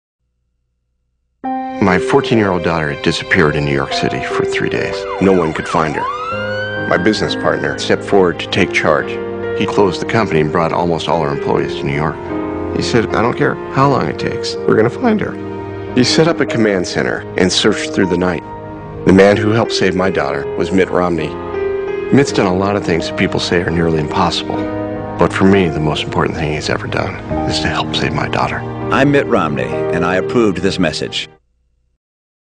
Another Example of Mitt Romney's Leadership
Despite the best efforts of some on the left to persuade otherwise, this ad from 2008 is entirely accurate

Mitt's character and method of handling a crises are both on display in this situation. Mitt Romney oozes presidential leadership out of every pore and our country will be blessed to have this man as our next president.